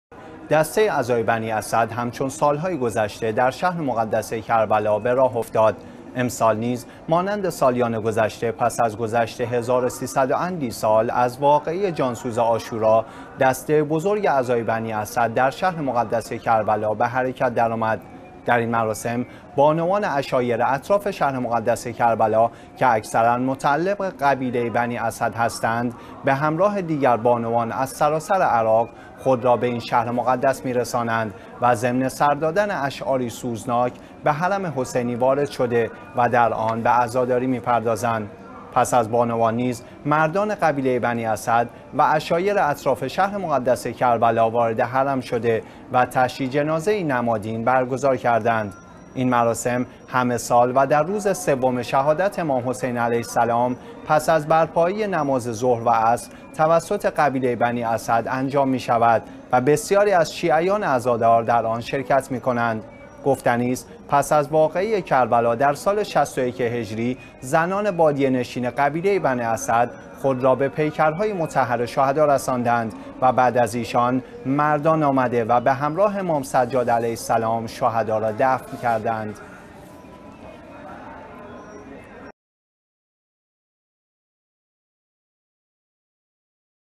13 محرم الحرام سالروز حرکت دسته عزای بنی اسد؛ قدیمی ترین دسته عزاداری شهر مقدس کربلا
دسته عزای بنی اسد همچون سال های گذشته در شهر مقدس كربلا به راه افتاد.
امسال نیز مانند سالیان گذشته پس از گذشت هزار و سیصد و اندی سال از  واقعه جان سوز عاشورا، دسته بزرگ عزای بنی اسد در شهر مقدس کربلا به حرکت در آمد.
در این مراسم، بانوان عشایر اطراف شهر مقدس کربلا که اکثراً متعلق به قبیله بنی اسد هستند به همراه دیگر بانوان از سراسر عراق، خود را به این شهر مقدس می رساندند و ضمن سر دادن اشعاری سوزناک به حرم حسینی وارد شده و در آن به عزاداری می پردازند.
پس از بانوان نیز، مردان قبیله بنی اسد و عشایر اطراف شهر مقدس کربلا وارد حرم شده و تشييع جنازه ای نمادين برگزار کردند.
این مراسم، همه سال و در روز سوم شهادت امام حسين عليه السلام، پس از بر پایی نماز ظهر و عصر، توسط قبیله بنی اسد، انجام می شود و بسیاری از شیعیانِ عزادار، در آن شرکت می کنند.
گفتنی است، پس از واقعه كربلا در سال ٦١ هجری، زنان بادیه نشین قبیله «بنی اسد» خود را به پیکرهای مطهر شهدا رساندند و بعد از ایشان، مردان آمده و به همراه امام سجاد علیه السلام؛ شهدا را دفن کردند.